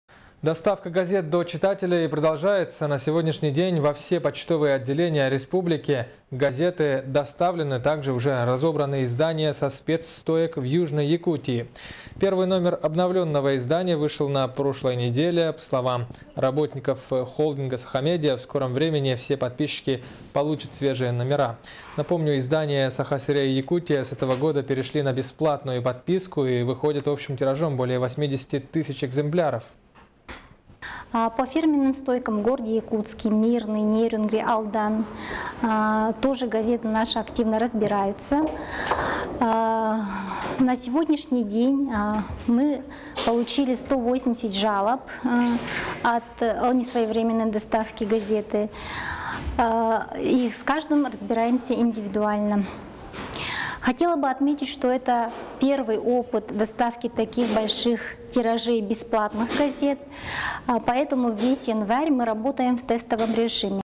Доставка газет «Саха сирэ» и «Якутия» до читателей продолжается
Официальный сайт NVK-ONLINE.RU - На сегодняшний день, во все почтовые отделения Якутии газеты доставлены, также уже разобраны издания со спец. стоек в Южной Якутии.